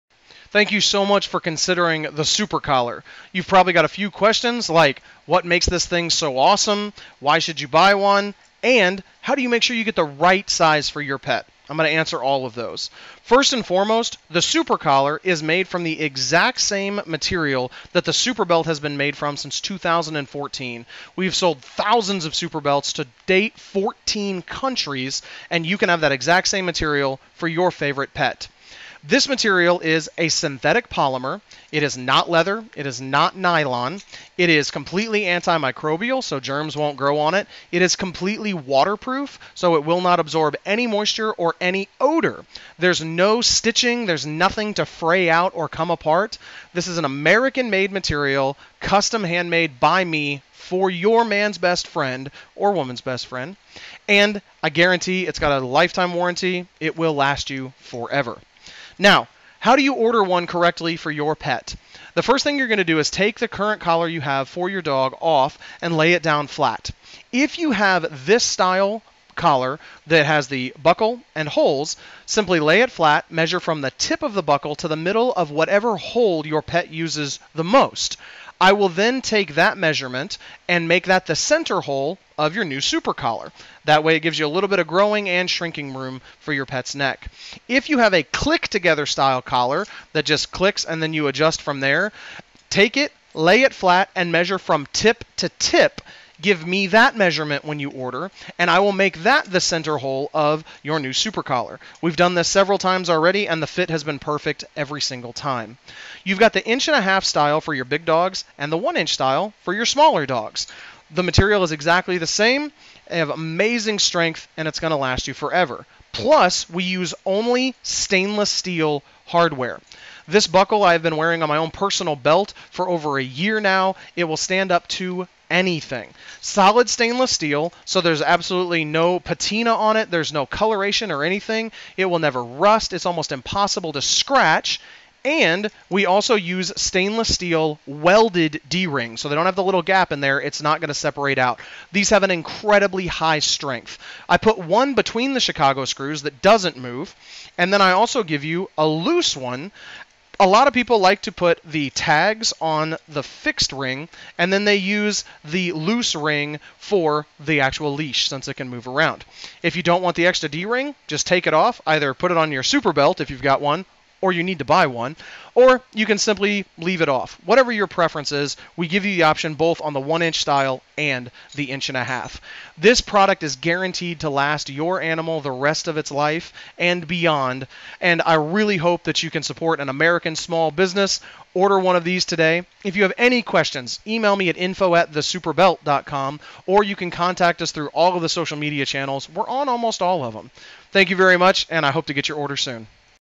The Super Collar : American Made, Indestructible Dog Collar
Unbreakable, American Made Dog Collar that's NOT Leather. Waterproof. Won't retain scent. Won't Break! Stainless Steel Hardware.